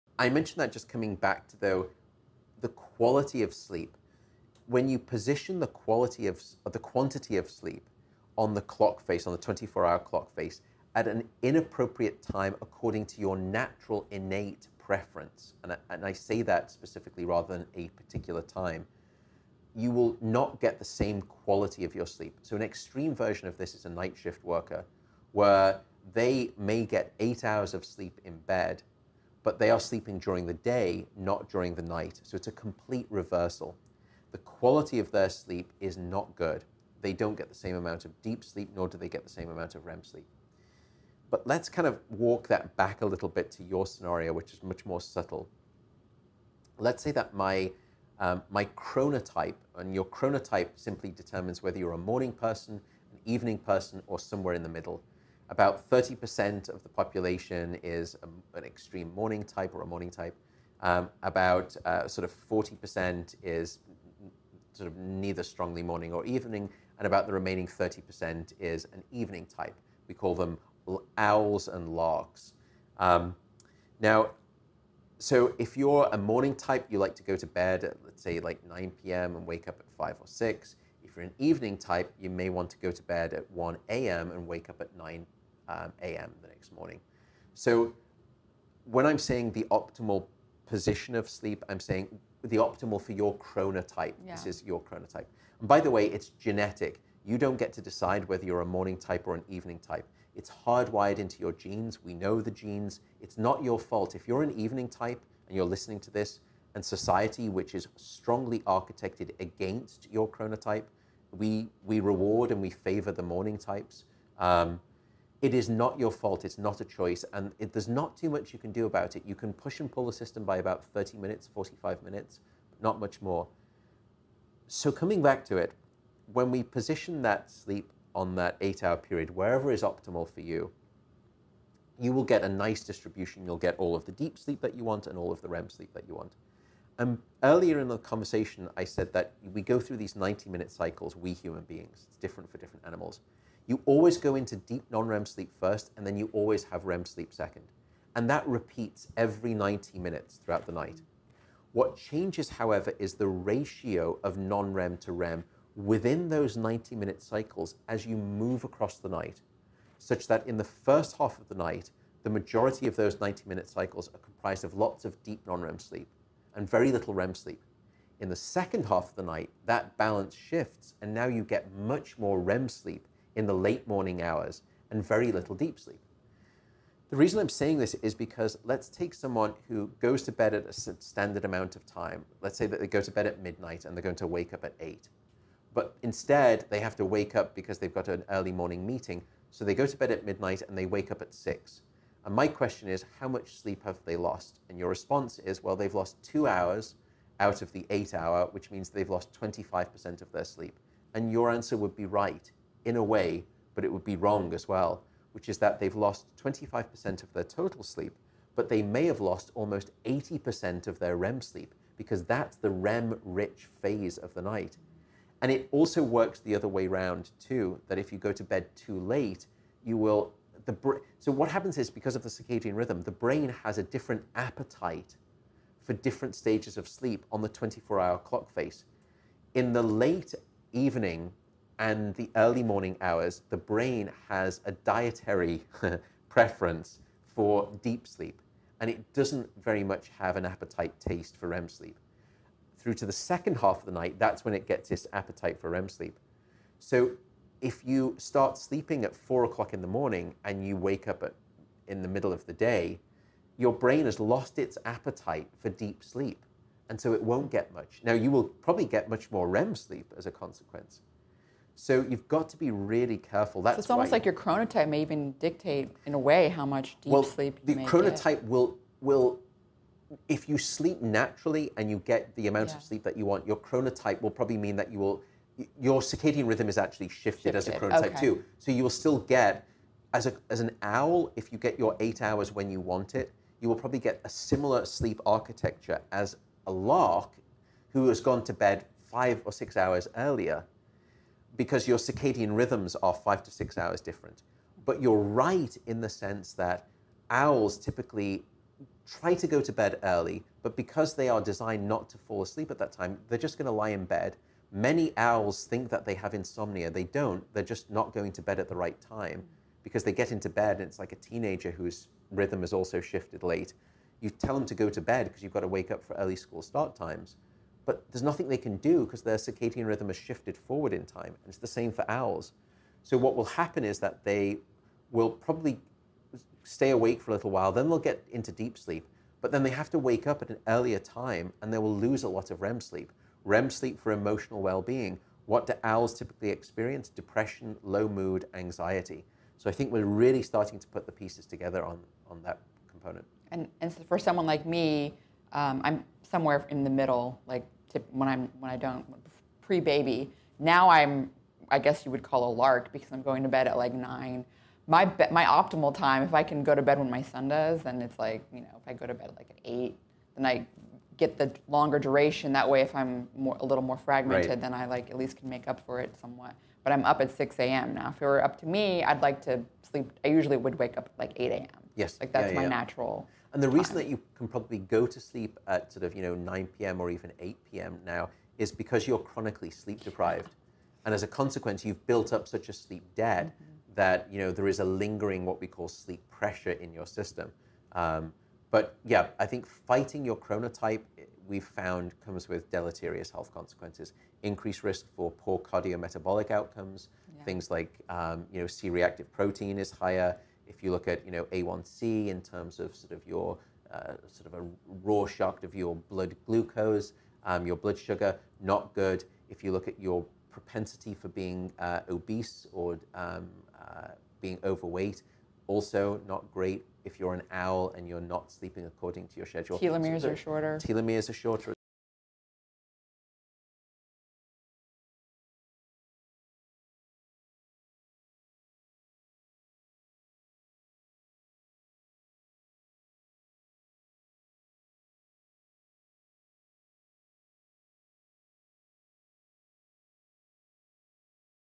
Chronotypes: Your natural propensity to be an early riser or night owl | Matthew Walker
A person's chronotype, their innate tendency to sleep at a particular time during a 24-hour period, is based strongly on genetically determined circadian rhythms. Disruption of a person’s chronotypic schedule can influence mood, productivity, and disease risk because it may interfere with the duration of their REM sleep, which is essential for emotional well-being. In this clip, Dr. Matthew Walker describes the different chronotypes and the risks associated with adopting a sleep pattern that is contrary to our natural type.

Original episode published on Feb 28, 2019